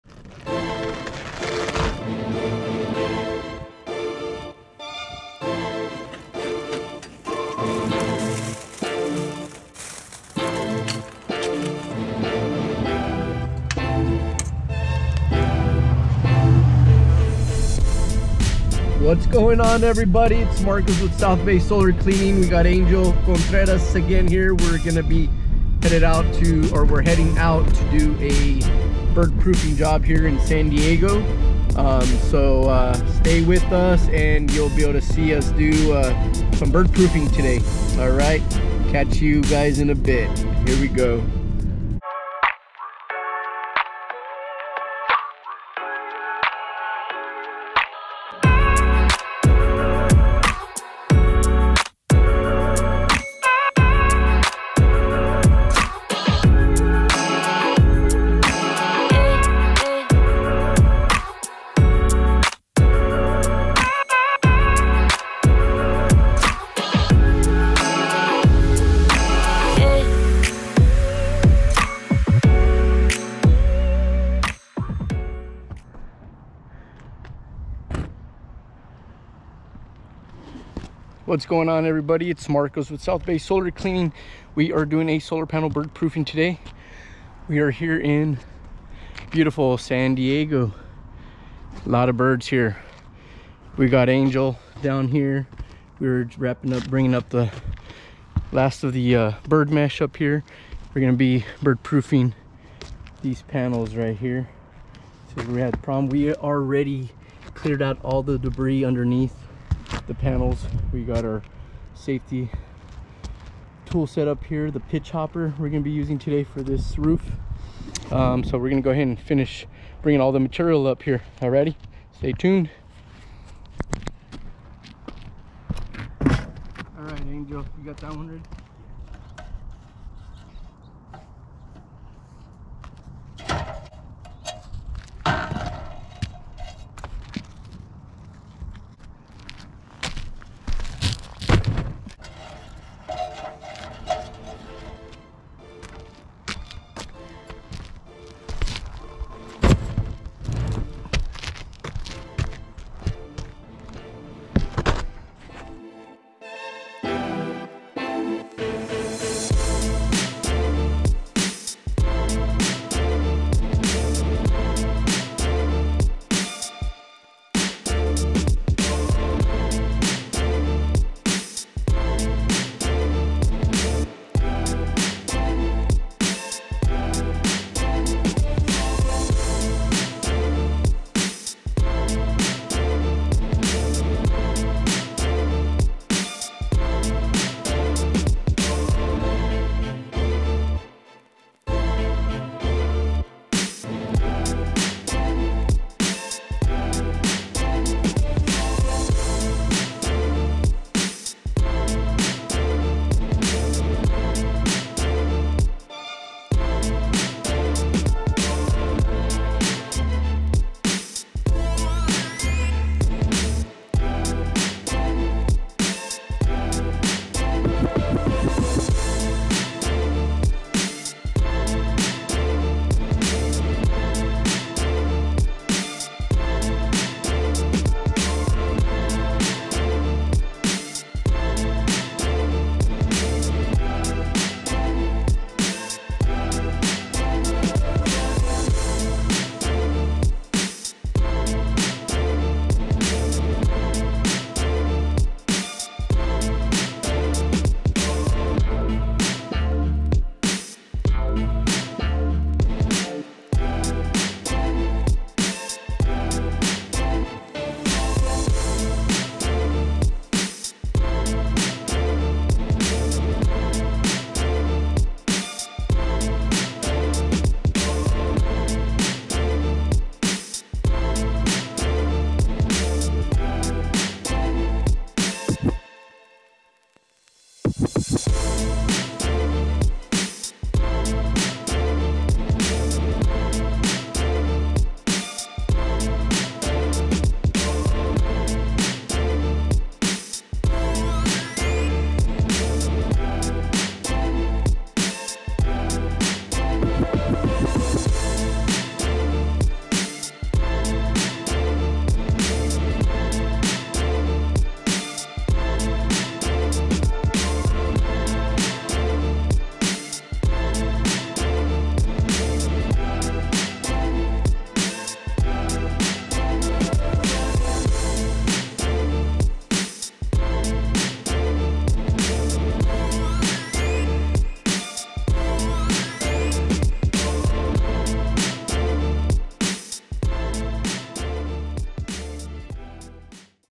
Solar Panel Bird Proofing Using The Pitch Hopper
Solar Panel Bird Proofing using the Pitch Hopper on a 12/12 roof.